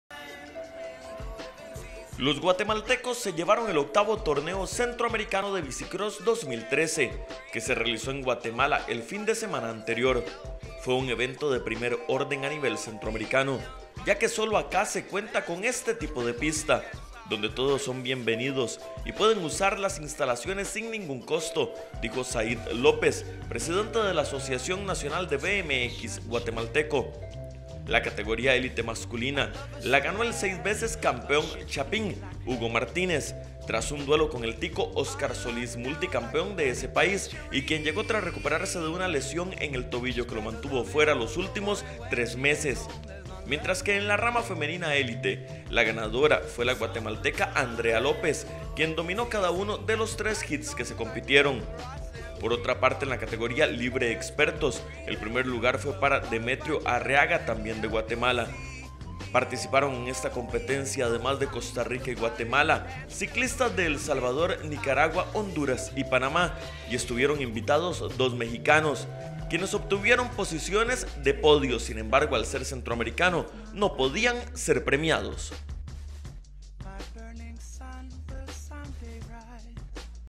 Guatemala ganó Torneo Centroamericano de Bicicrós
Los guatemaltecos se llevaron el octavo Torneo Centroamericano de Bicicrós 2013, que se realizó en Guatemala.
La categoría élite masculina, la ganó el seis veces campeón chapín Hugo Martínez, tras un duelo con el tico Óscar Solís.